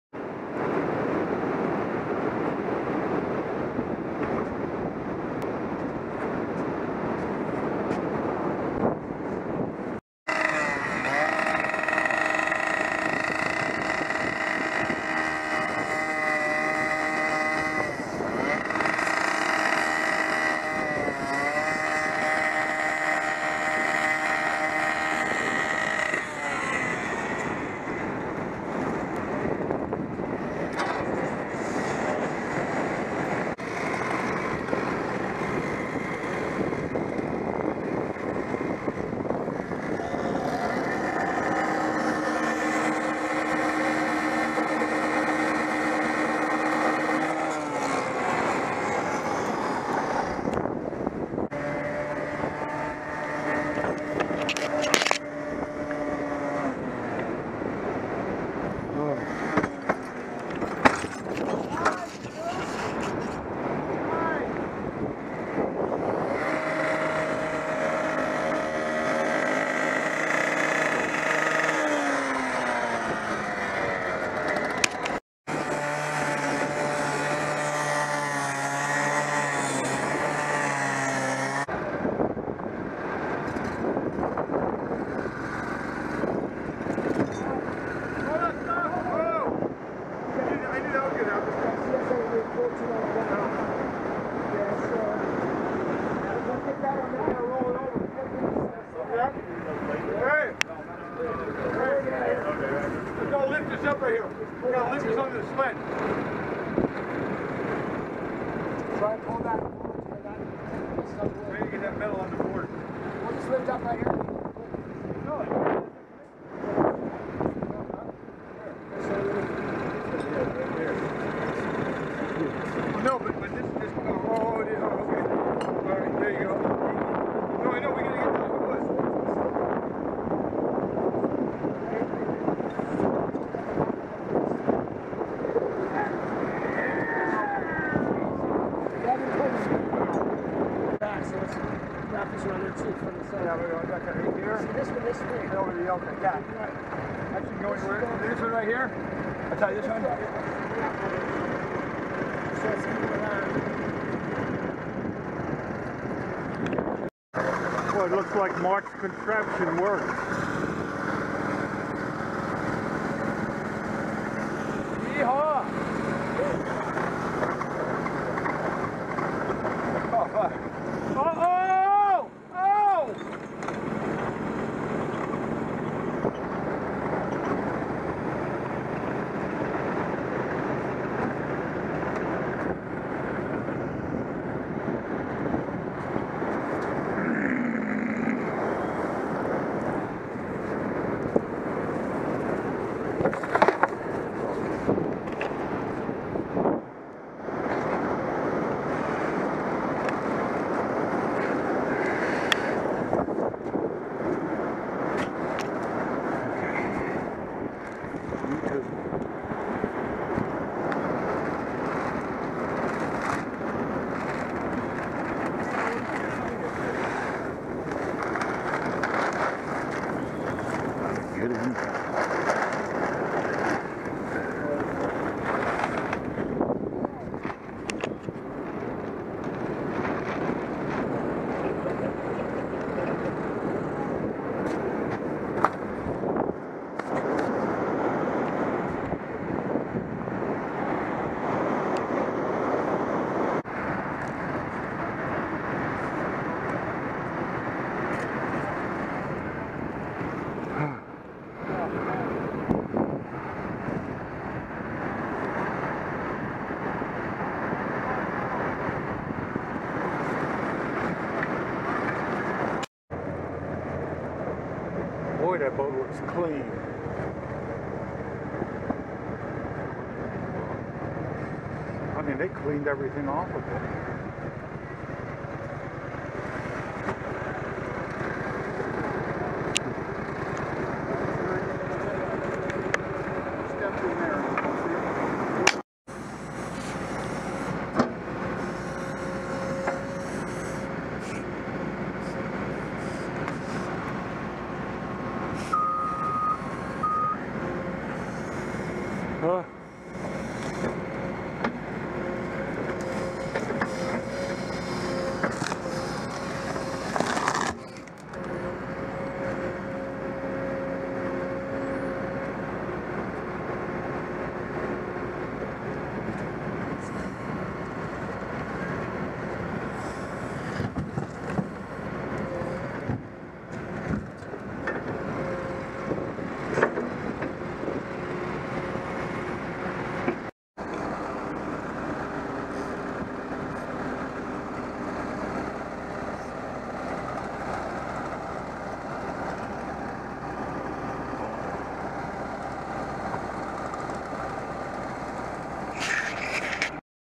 Deconstruction of the Seastar Kehoe beach Point Reyes, CA February 26, 2022
On the exterior, the crew takes to the vessel with gasoline powered circular saws, which we refer to as the "Cutoff Saw" and generator powered Sawzall.  The Gurney poles, the mast, and flood lights, the hatches, the generator, and the stack will need to be removed. That looks like Dion up there looking very satisfied with himself as he kicked the un-stayed mast down. Next is the house which will be cut right down to the deck.